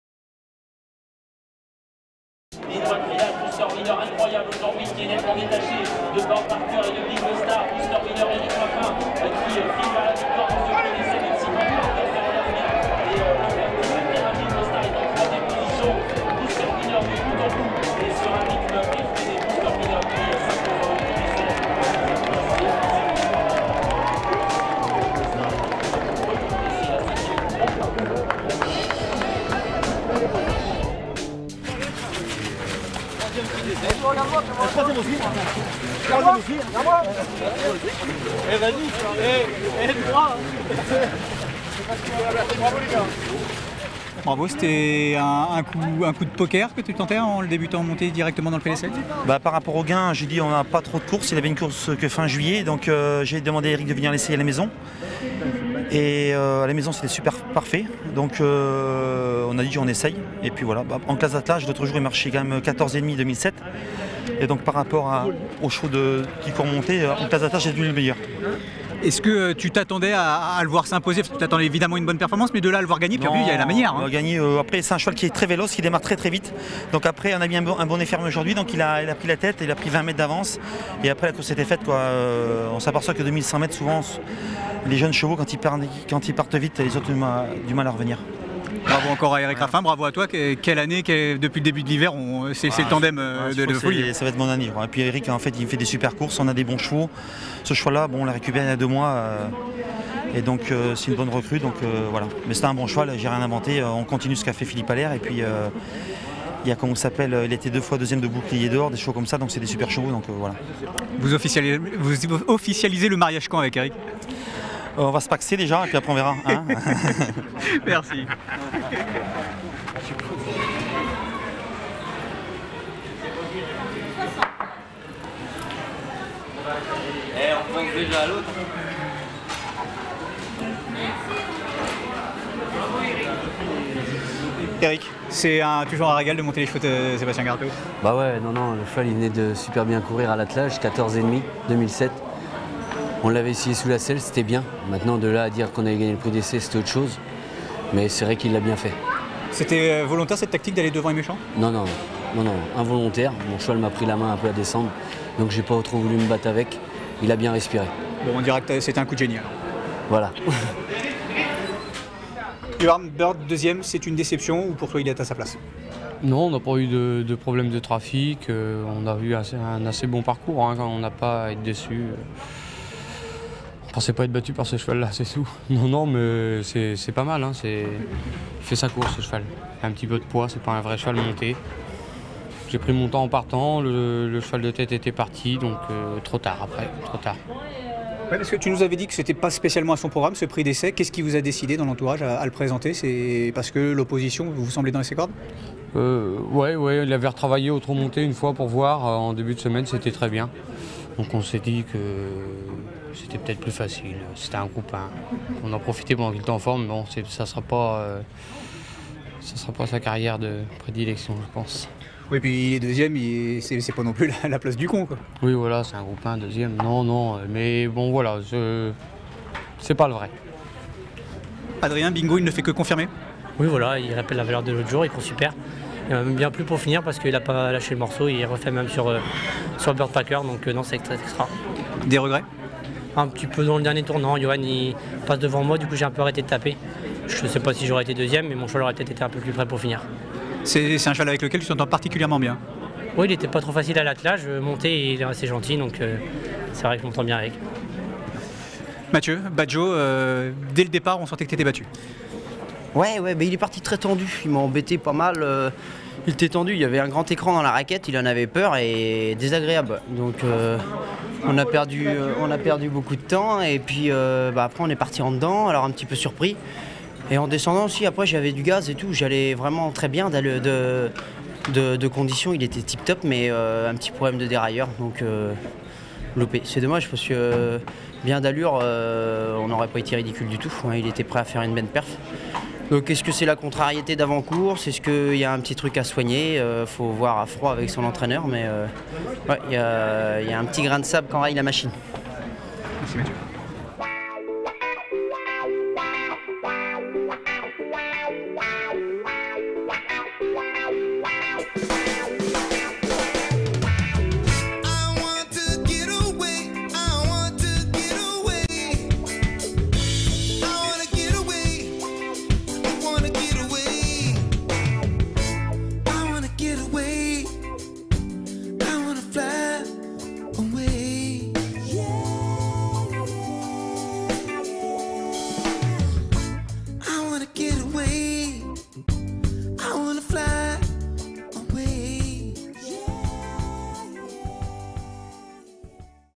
Coup d'Essai, coup de Maître pour BOOSTER WINNER - A2TURF.COM
L'entourage de BOOSTER WINNER tentait un coup d'Essai en débutant directement sous la selle dans un groupe I. Coup de maître puisque le fils de Love You a survolé les débats dans une course marquée par des disqualifications.
La phase finale et les réactions des premiers ainsi que des battus.